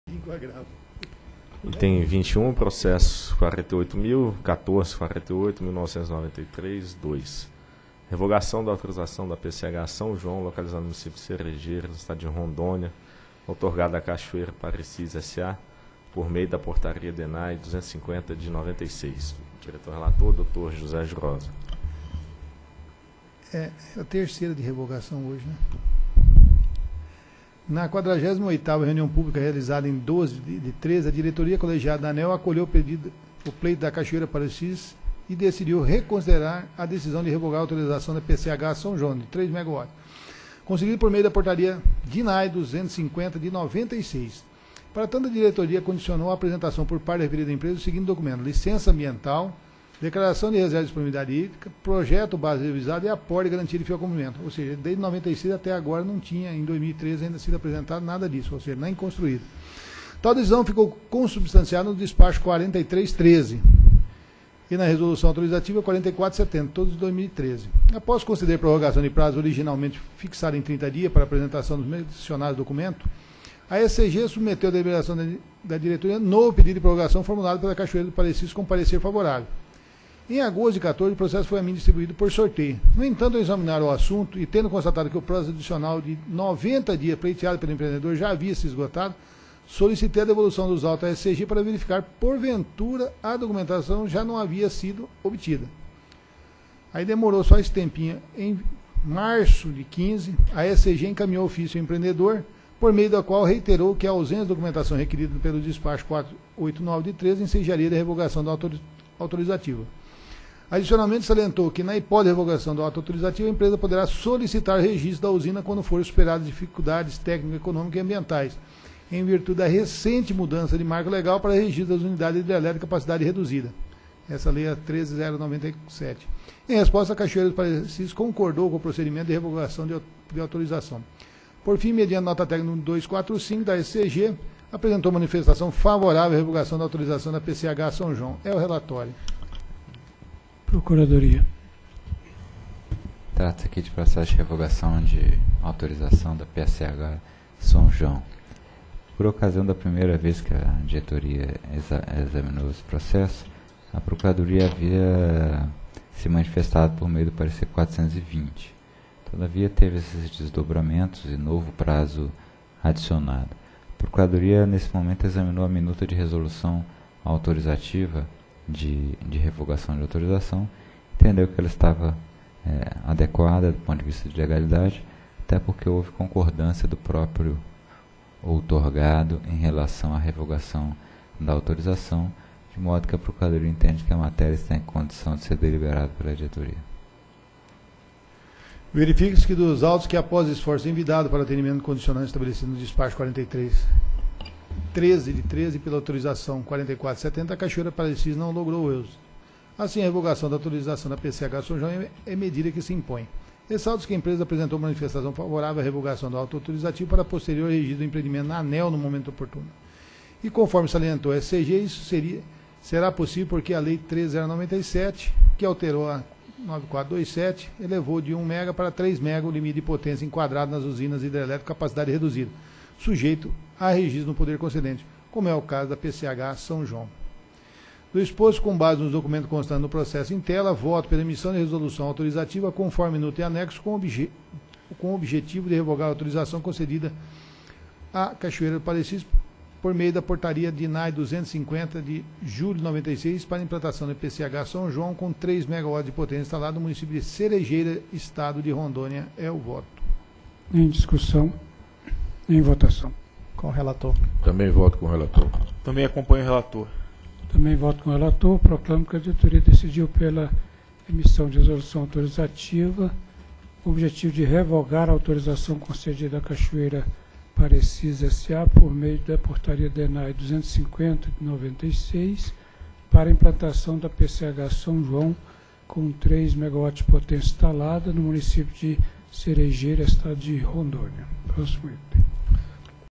Item 21 - 13ª Reunião Pública Ordinária ANEEL 22/04/2015
Item 21. Processo: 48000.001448/1993-02. Assunto: Revogação da autorização da Pequena Central Hidrelétrica – PCH São João, localizada no município de Cerejeira, estado de Rondônia, outorgada à Cachoeira Parecis S.A. por meio da Portaria DNAEE nº 250/1996. Relator: Diretor José Jurhosa Junior.